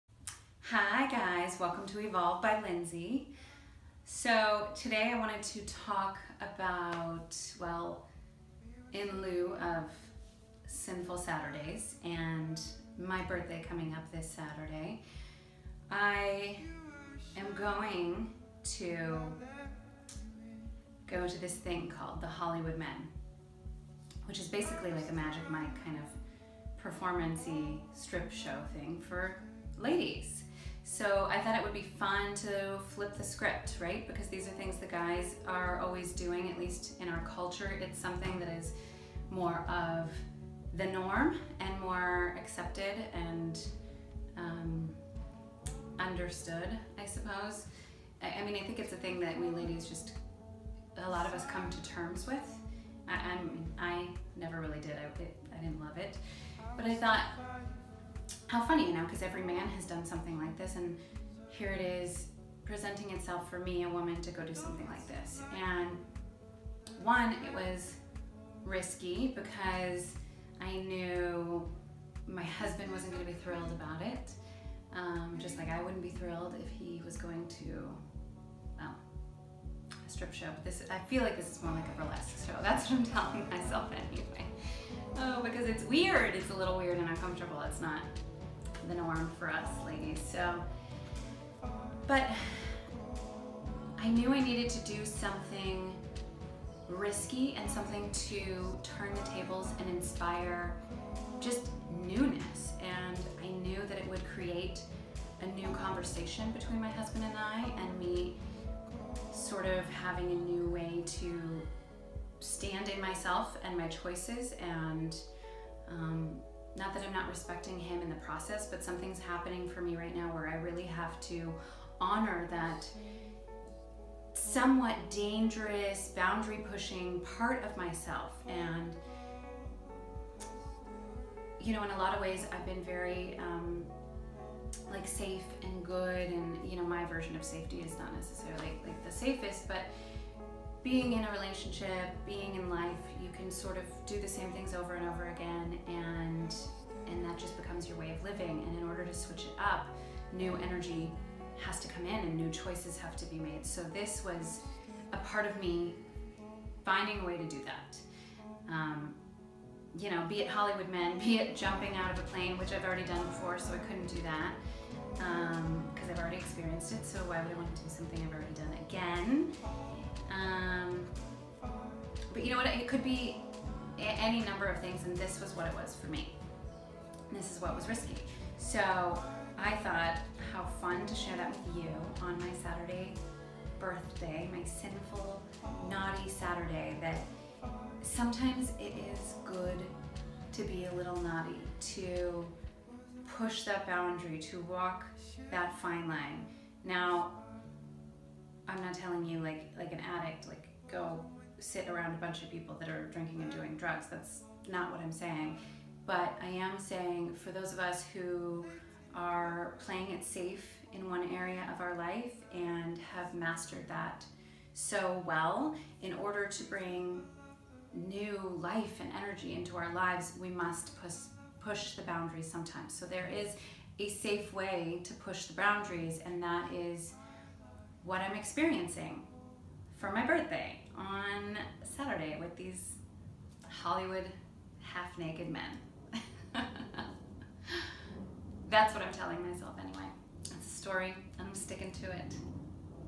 NAUGHTY ADVENTURE
Lindsey discusses going to The Hollywood Men for her Birthday and how that pushes the boundaries of her life, her relationship and her growth.

For More Visit EvolveByLindsey.com